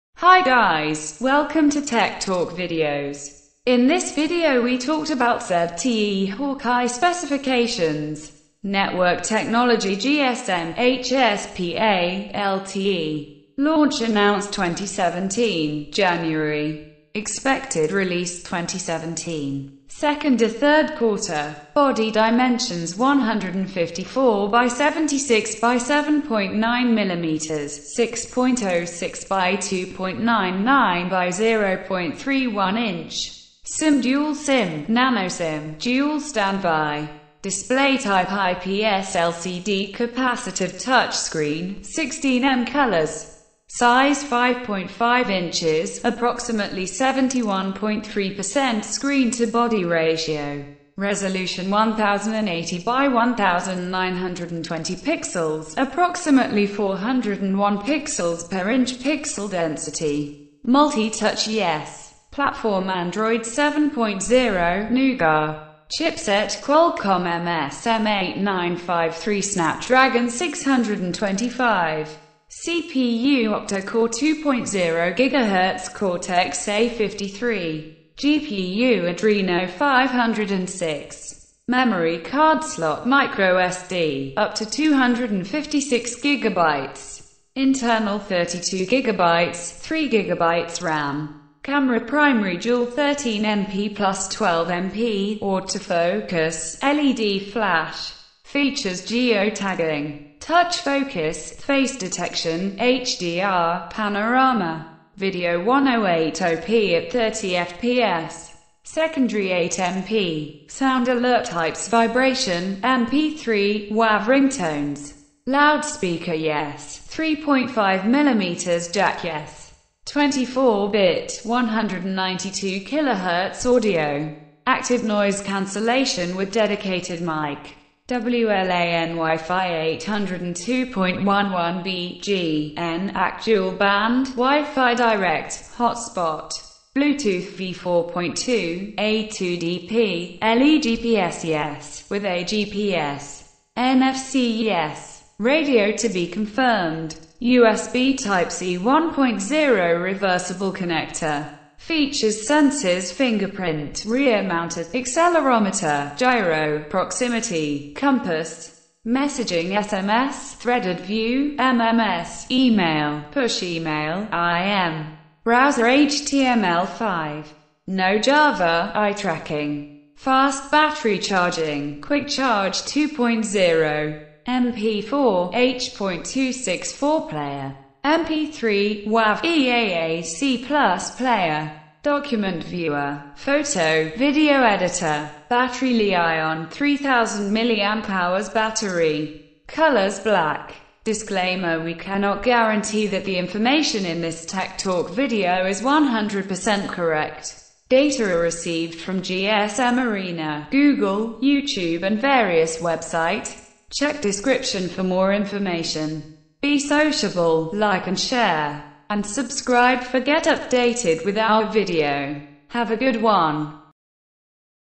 ZTE Hawkeye Specification || Product Details || TechSpecT
Network  Technology  GSM / HSPA / LTE.
Launch  Announced  2017, January.
 Expected release 2017, 2nd or 3rd quarter.
Body  Dimensions  154 x 76 x 7.9 mm (6.06 x 2.99 x 0.31 inch).
Platform    Android 7.0 (Nougat).
Camera  Primary  Dual 13 MP + 12 MP, autofocus, LED flash.
Features  Geo-tagging, touch focus, face detection, HDR, panorama.
Video  1080p@30fps.
Secondary  8 MP.
WLAN  Wi-Fi 802.11 b,g,n,ac, dual-band, WiFi Direct, hotspot.
Bluetooth  v4.2, A2DP, L.E.
GPS  Yes, with A-GPS.
NFC  Yes.
USB  Type-C 1.0 reversible connector.